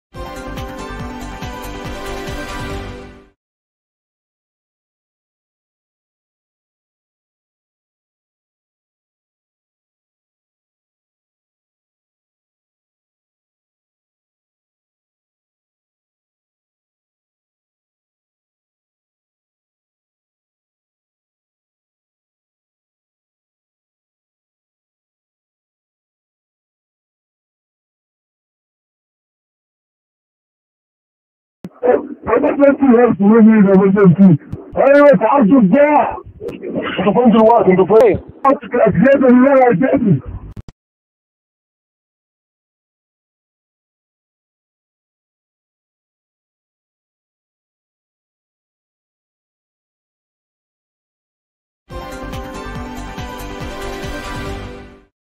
Milano, fermati 10 egiziani accusati di immigrazione clandestina
MILANO (ITALPRESS) -  La Polizia di Stato di Milano nelle prime ore di stamattina ha dato esecuzione a un Decreto di fermo di indiziato di delitto disposto dalla Procura della Repubblica di Milano - Direzione Distrettuale Antimafia - a carico di 10 soggetti di origine egiziana, indagati a vario titolo per i reati di associazione per delinquere, favoreggiamento dell’immigrazione clandestina nonché esercizio abusivo dell’attività creditizia. In particolare, l’attività d’indagine ha consentito di raccogliere indizi di reato in ordine alla presenza nel capoluogo lombardo di un sodalizio, avente carattere della transnazionalità e composto da più soggetti di origine egiziana, dedito al trasferimento illegale di migranti, sempre di nazionalità egiziana, sul territorio nazionale e su quello di altri Paesi europei, tramite imbarcazioni salpate dalle coste libiche.(ITALPRESS)

trl/gsl